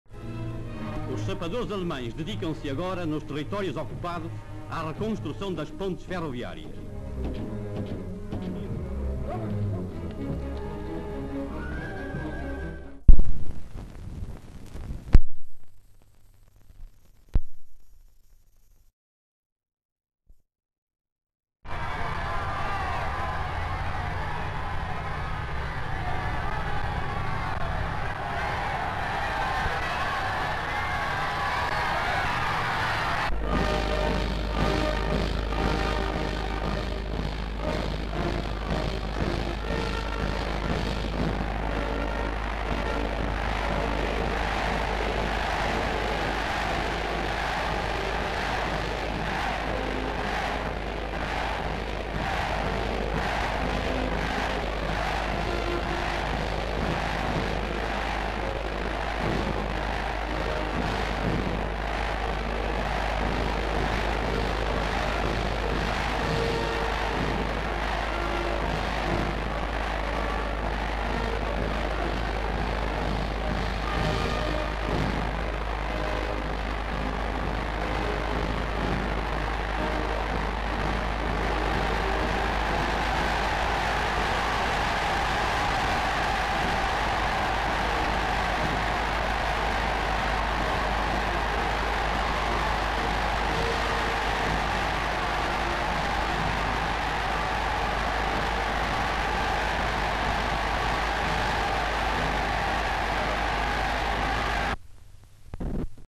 Hitler In Graz (1938)
Unconnected piece - Men work on some kind of construction project.

Spacing

Graz, Austria.

Motorcade arrives carrying Adolf Hitler. Large crowds cheer as he passes by.

Inside a factory or hanger people have gathered to see the Fuhrer. Hitler walks through crowd and accepts flowers. Various CUs of Austrian showing their support with Nazi salutes.

N.B. Sound unclear - appears to have been over recorded.
 FILM ID:560.2